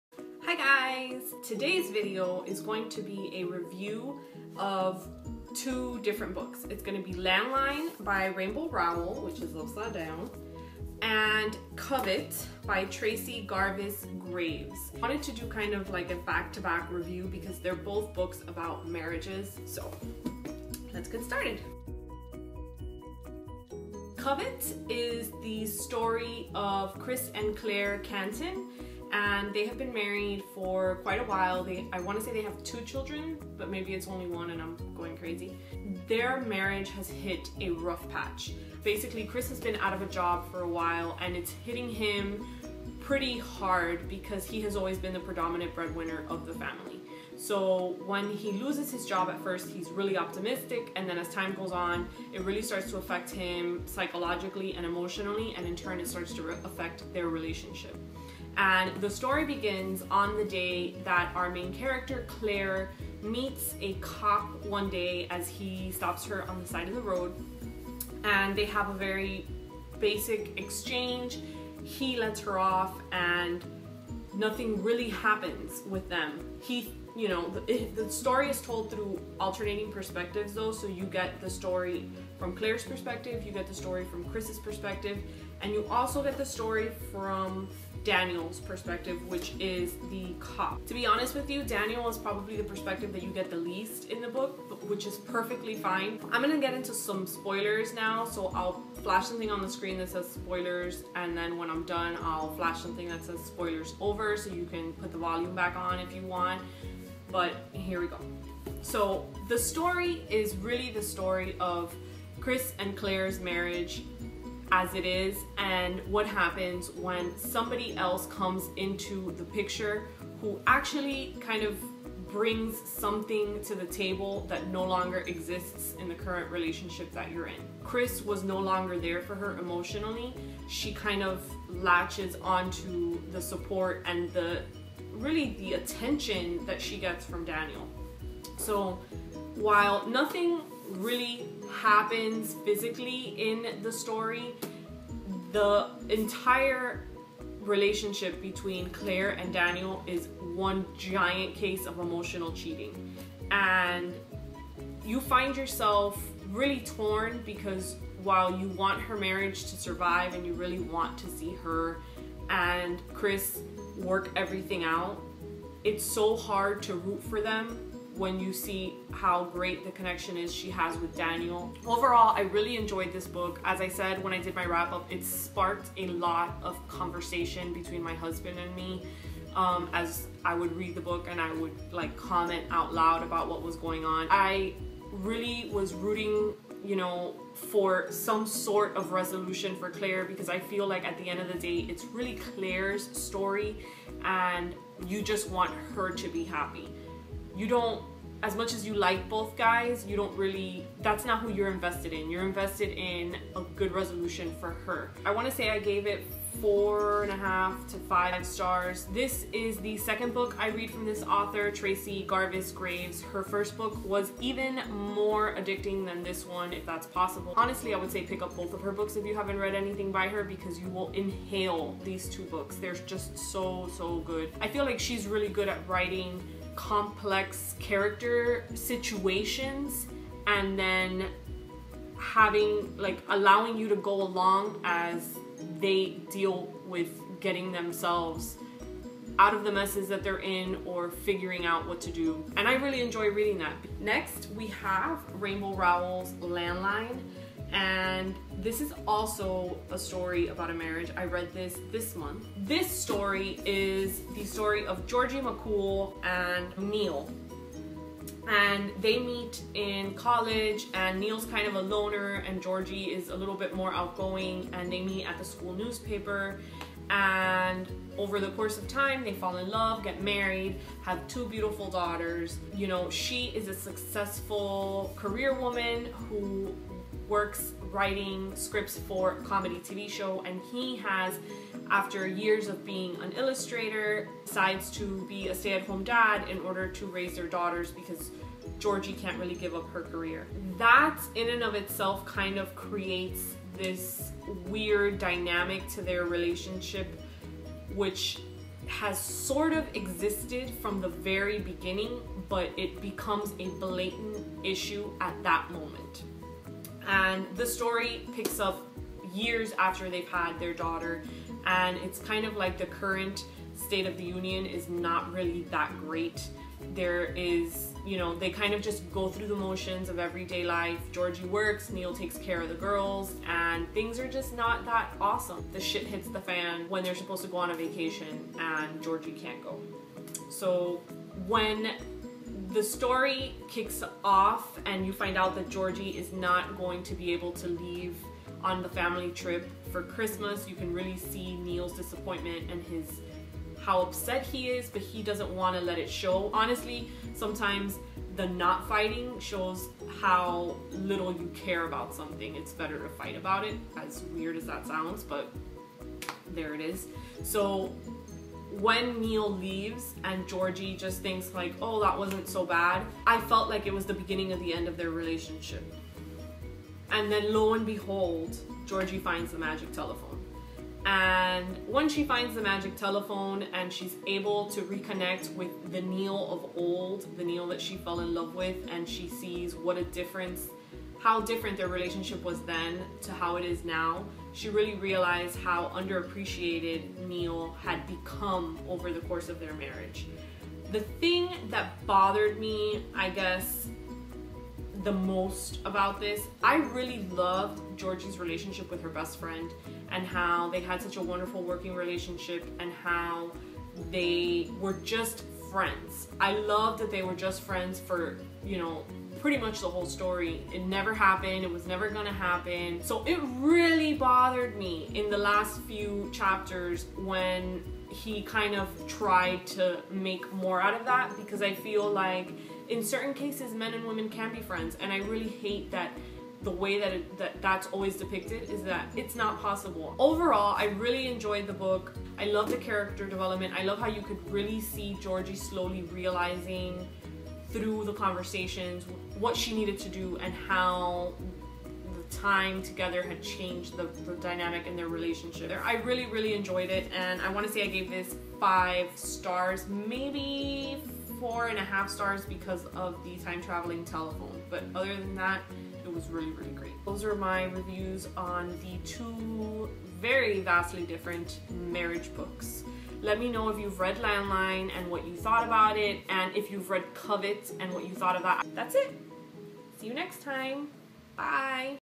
Landline and Covet Reviews II 2014
I wanted to review these both together because they each took a different approach to troubled marriages.